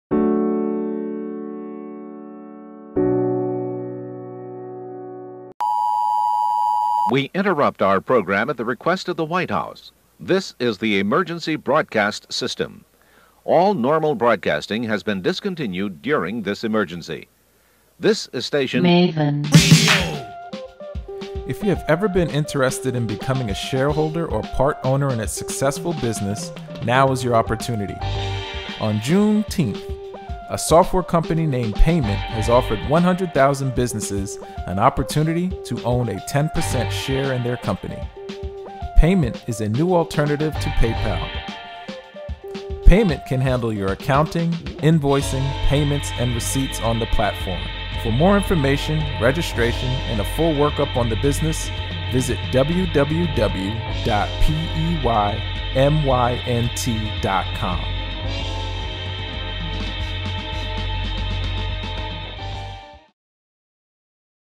Maven Radio - Peymynt Investment
Peymynt is giving 100,000 businesses an opportunity to invest in their company for a 10% share for free!  Go to their instagram page follow, forward and tag them and then go to their website and sign up for free,